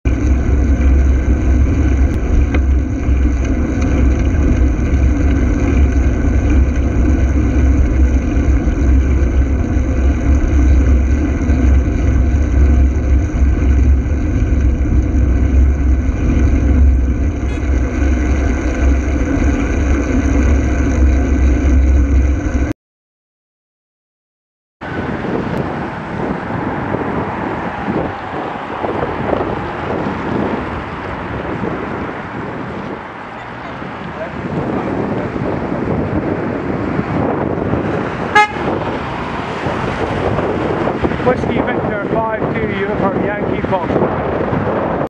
WV52UYF what a genius
We've got a real genius here, thinks the car that's indicating right is going to overtake me on the corner and not turn right so he follows them thinking he'll get past me as well. Gets a bit of a shock when it turns out that the car, which is indicating right approaching a right turn, turns right. As he passes me he honks and gestures, obviously holding me responsible for his poor driving.

Thursday 10th December 2015 approx 17:30